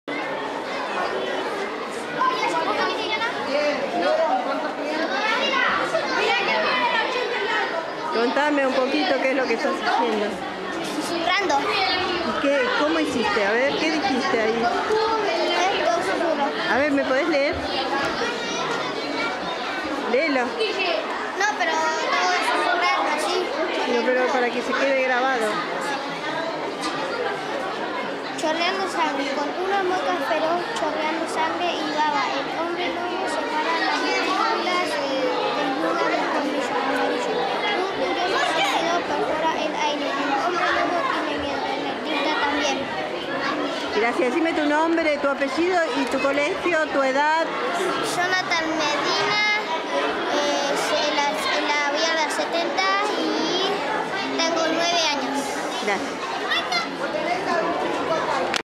YONATAN MEDINA, ALUMNO DE LA ESCUELA N° 70 ( SUSURRO)
EXITOSO ARRANQUE DEL PRIMER ENCUENTRO DE LITERATURA Y ARTE PARA NIÑOS Y JÓVENES

Alumnos de más de 70 escuelas del municipio expusieron sus trabajos en la Casa municipal de la Cultura. El intendente Darío Giustozzi visitó los stands, escuchó a los "Susurradores" que  leyeron cuentos y leyendas y se llevó el cariño de los chicos, que contaron con un espacio para fomentar su imaginación y expresar su vocación artística.

Arrancó con éxito el Primer Encuentro de Literatura y Arte Infanto Juvenil de Almirante Brown, que se desarrolla como cierre del Mes de Borges en la Casa municipal de la Cultura.

Duendes, hadas, dragones, brujas y demás personajes de los cuentos y las leyendas cobraron vida a través del trabajo expuesto por alumnos de más de 70 escuelas del municipio, que contaron con un espacio para fomentar su imaginación y expresar su vocación artística.

El encuentro, promovido por el gobierno municipal y la Asociación de Docentes jubilados de Almirante Brown, contó con la presencia del intendente Darío Giustozzi; que visitó los stands, escuchó a los "Susurradores" que leyeron cuentos y leyendas, y se llevó el cariño de los chicos.

Estuvieron presentes, además, el secretario de Educación y Cultura, escribano Jorge Herrero Pons; la directora de Promoción Cultural, Sandra Agis; el subsecretario de Educación; Jorge Ameal; la presidente de la Asociación de Docentes jubilados de Almirante Brown, Marta Freixies y la inspectora Nora Saporitti.

"En el mes del Homenaje a Borges, le hacemos un homenaje a la lectura y a las expresiones artísticas. Por eso estamos muy contentos con el éxito de este primer encuentro que contó con la participación de 74 escuelas. La importancia de los estímulos que genera la lectura es irreemplazable. Esos estímulos se convierten en conocimiento y disparan a su vez la imaginación y el arte, que es lo que impulsamos desde el municipio continuamente a través de variados programas y talleres", expresó Giustozzi sobre el encuentro, que contó con exposiciones de libros, muestra de trabajos escolares, charlas con escritores, libreros y editores, y conferencias a cargo de especialistas.

"Borges estaba orgulloso de todo lo que leyó a lo largo de su vida, porque gracias a esto obtuvo el conocimiento que le sirvió para enriquecer su gran obra", agregó el intendente y destacó que "los homenajes deben ser activos, en este caso estimulando la lectura, la creatividad y las relaciones genuinas y directas, vínculos que no pueden ser reemplazados por la tecnología digital".

"Los chicos están orgullosos con sus trabajos y los cuentos que leyeron; y están felices de poder mostrar todo lo que aprendieron y realizaron este año junto a sus maestros", indicó asimismo la presidente de la Asociación de Docentes jubilados de Almirante Brown, quien agradeció poder contar con un espacio tan importante como la Casa de la Cultura para que se desarrolle la actividad. "Creemos que el estímulo de la imaginación posibilita generar cambios y ayuda a la resolución de conflictos", agregó.

El encuentro continuará mañana -sábado 24- de 10 a 17 en Esteban Adrogué 1224, Adrogué, con entrada libre y gratuita.